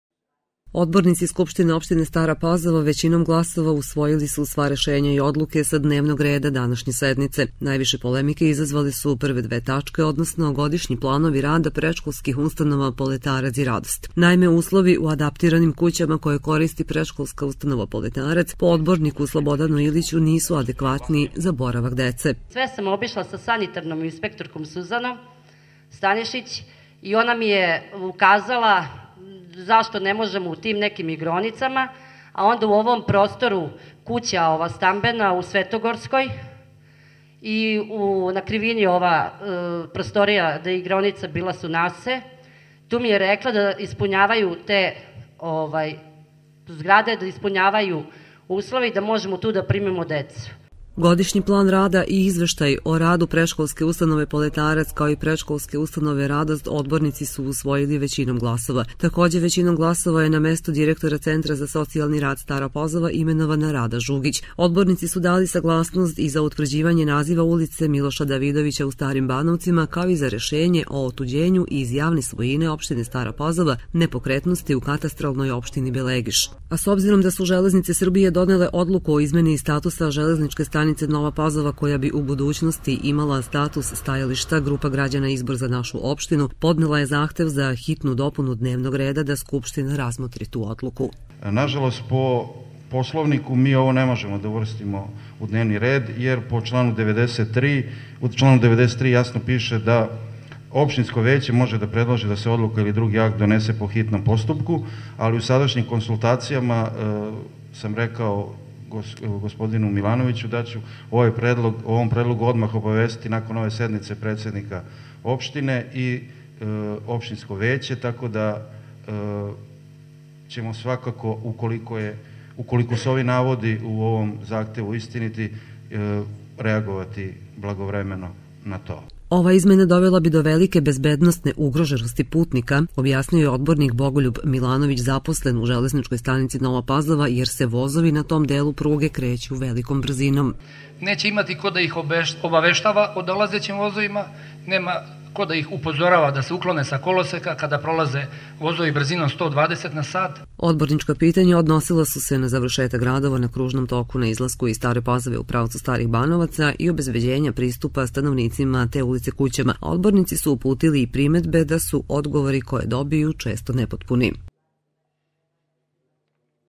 22. sednica Skupštine opštine Stara Pazova
Danas je održana sednica SO Stara Pazova. Pred odbornicima  se našlo šest tačaka, a većina se ticala rada ustnova čiji je osnivač SO.